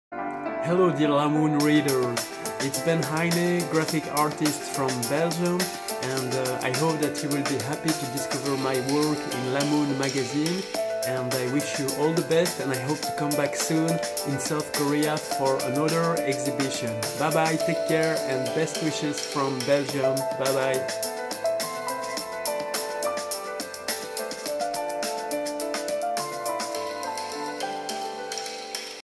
Ben Heine
The founder of Pencil Vs Camera and Digital Circlism, a Belgian artist Ben Heine meet La Moon Magazine.